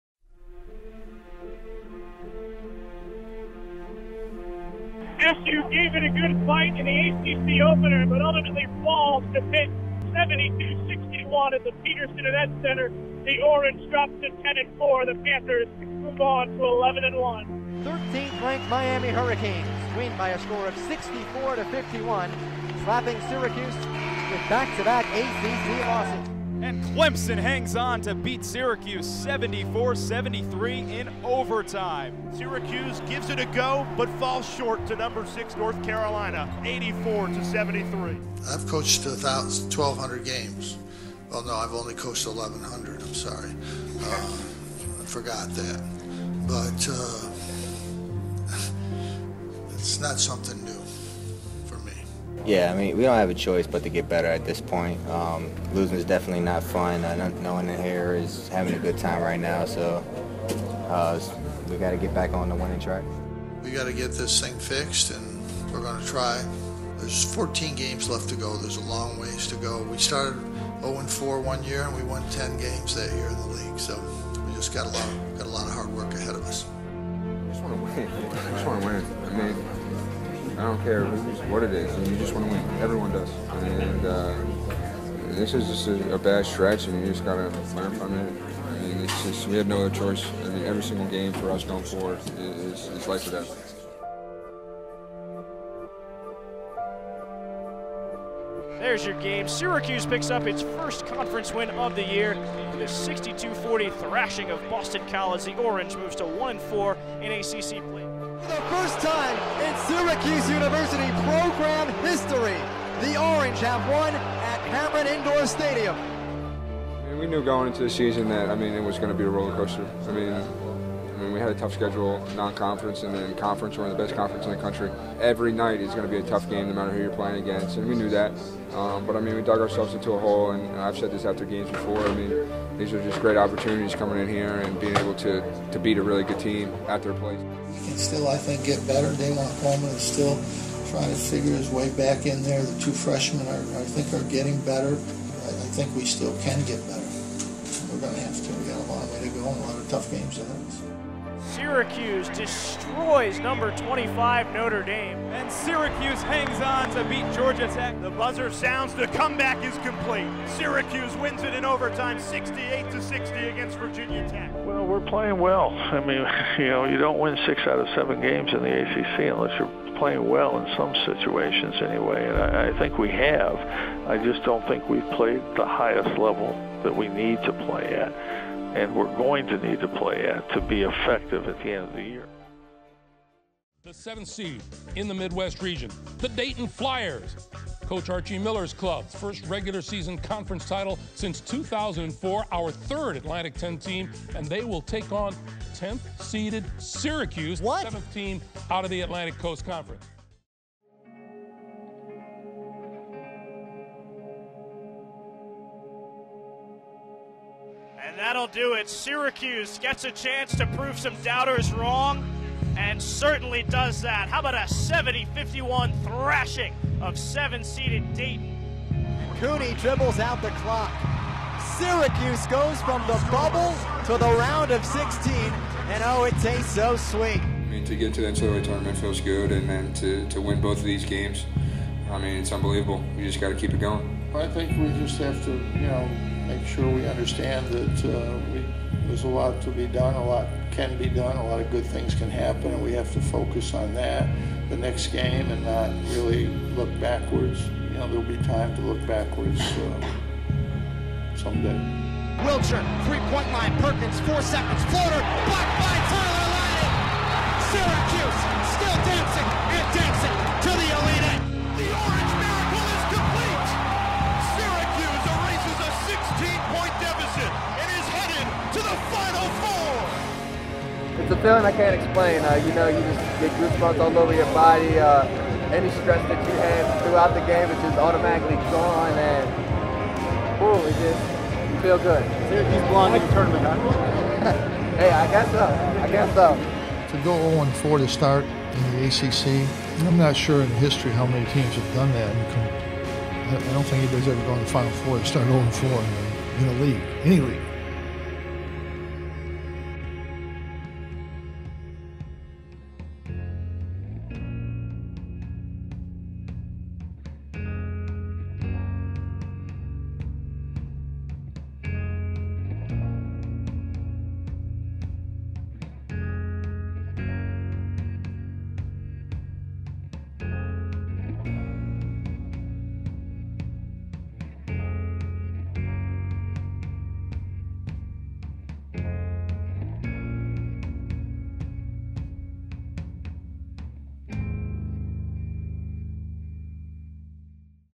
0- 4 to Final 4
The 2015-16 Syracuse men's basketball team fell to the barrel of the ACC after four straight conference losses. Listen through the voices of Coach Boeheim and seniors Trevor Cooney and Michael Gbinije about the story of what once looked like a lost season eventually became one of the greatest runs in program history. (Photo Credit: AP)